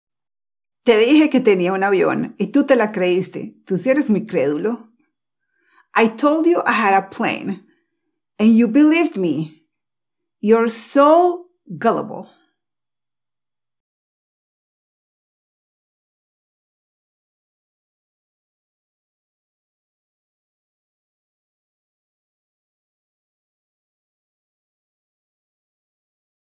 Vocabulario: You're so GULLIBLE!
Sigamos aumentando nuestro vocabulario.  Cuando alguien te dice, "Eres tan crédula" "You're so gullible"

I wish you a great day!